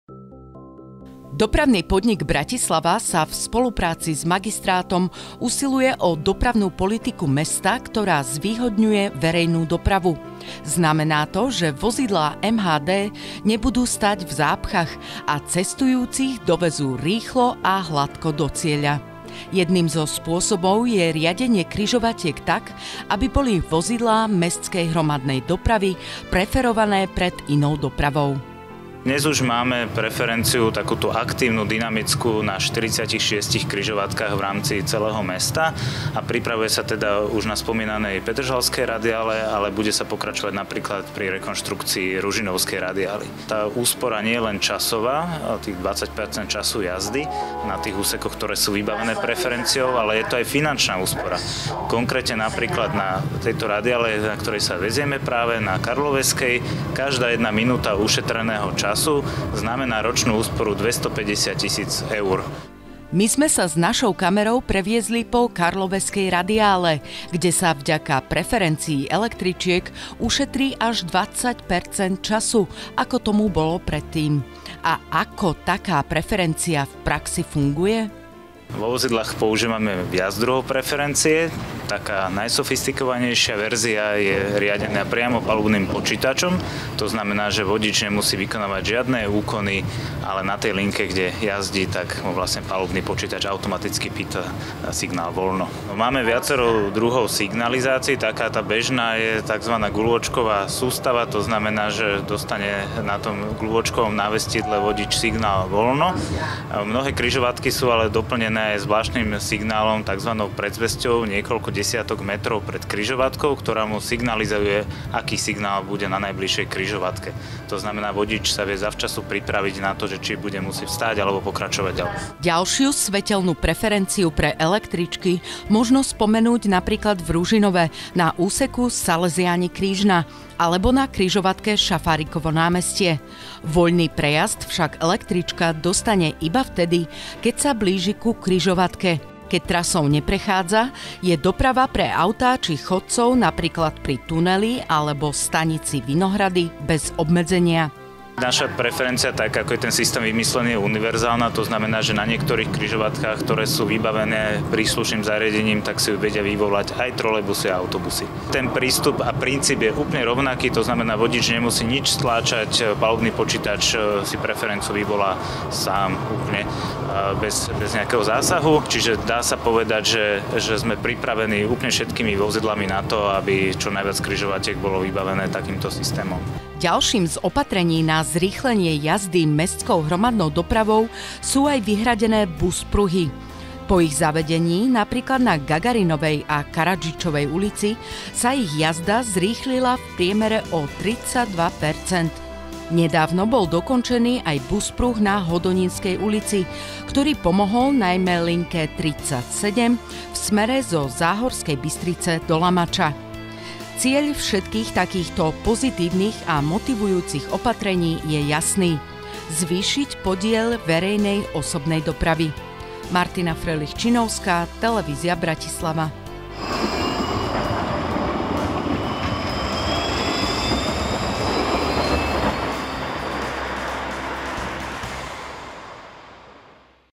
8.11.2024 | Ako funguje preferencia MHD v praxi?
Ak chce mestská hromadná doprava prilákať viac cestujúcich, musí mať viac výhod, ako individuálna doprava. Jednou z dôležitých výhod je presadzovanie tzv. BUS pruhov a ďalším riešením je riadenie križovatiek tak, aby boli vozidlá MHD preferované pred inou dopravou. Poďte sa s nami pozrieť, ako to v praxi funguje.
*K5766